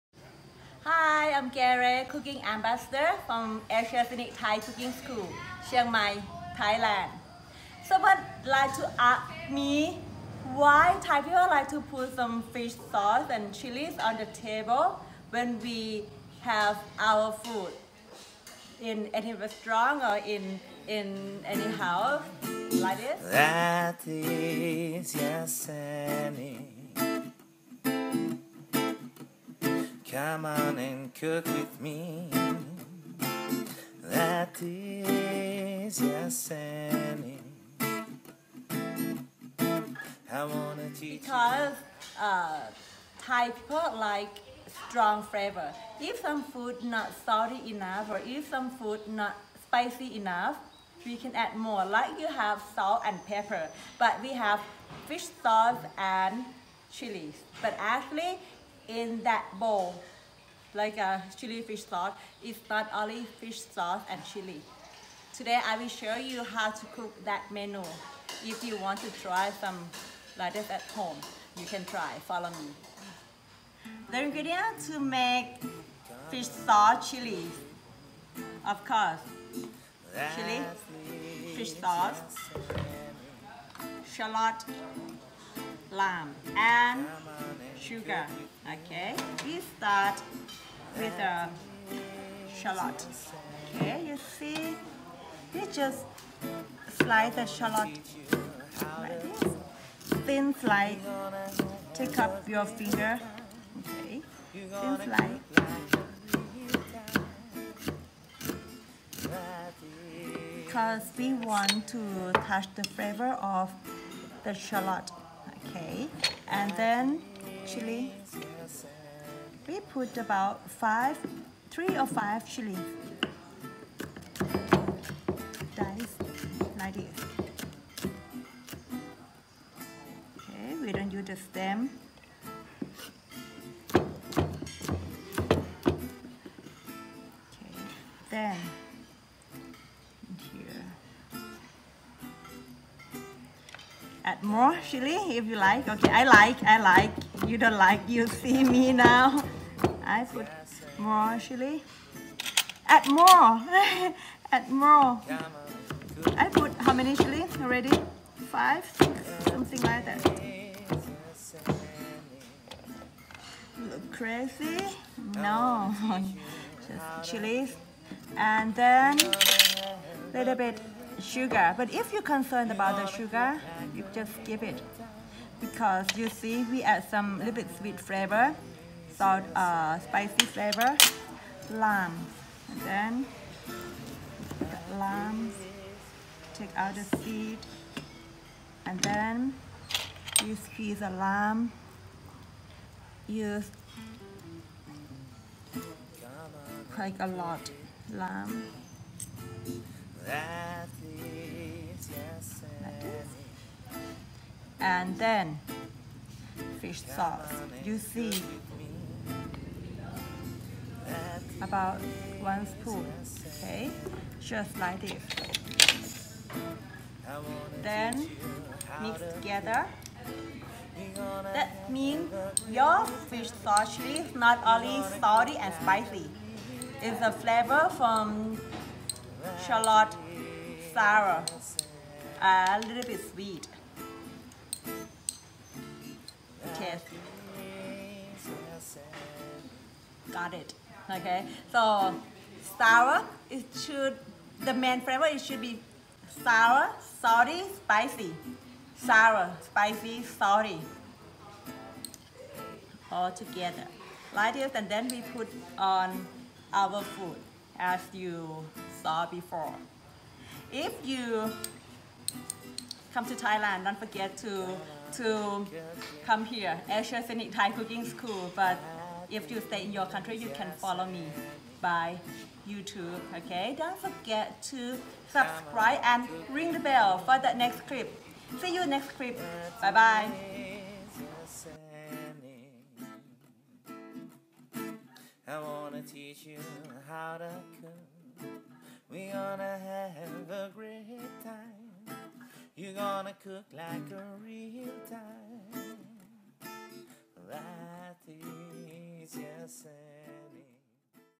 Chilli with fish sauce recipe Thai style | Asia Scenic Thai Cooking School
Easy recipe but big flavor for Thai people.

Ingredients : Chilli , fish sauce , Shallot , sugar
Flavor : sour - salty - spicy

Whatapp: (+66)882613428